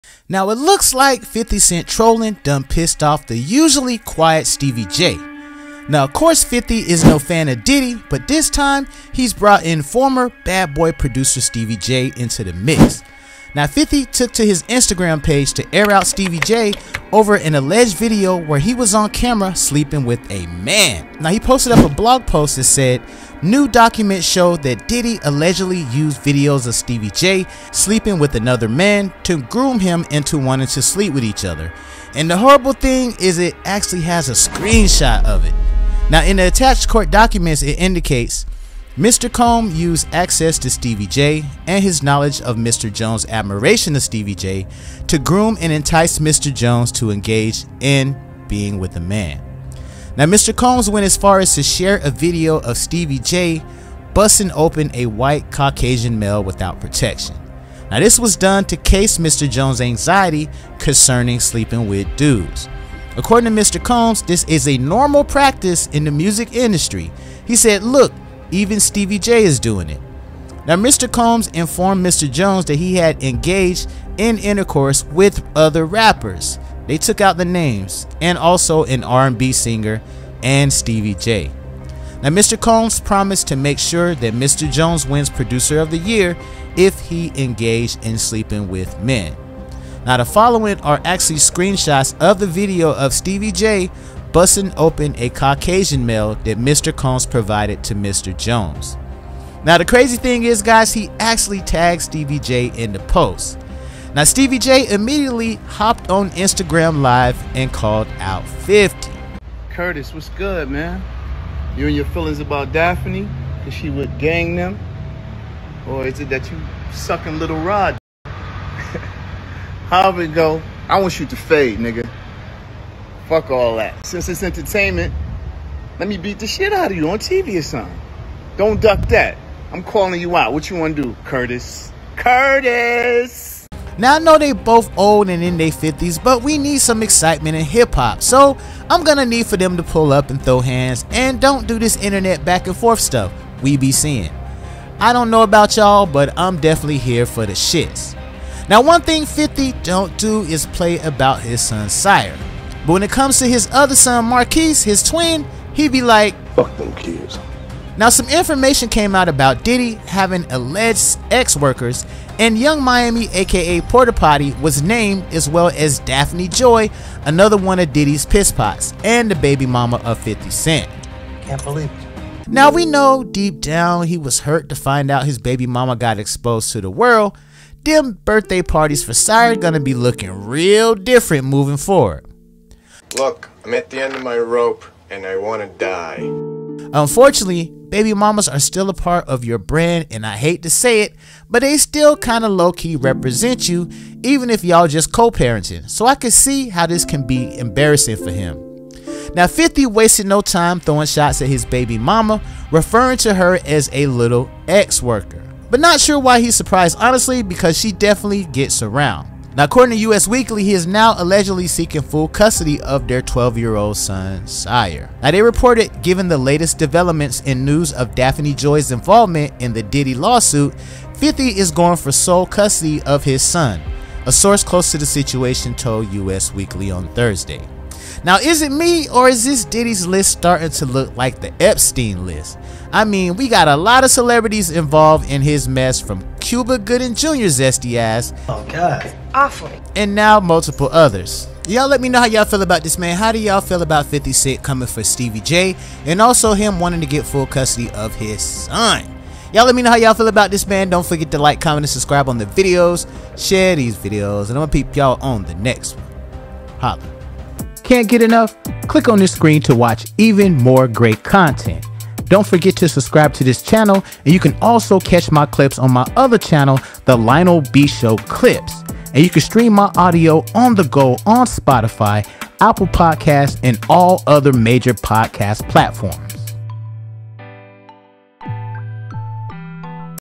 50 Cent Leaks Stevie J G*Y P*RN TAPE PICS, Stevie J Wants To F1GHT!
Stevie J and 50 Cent get into it over Diddy's Raid at his homes in Miami and L.A.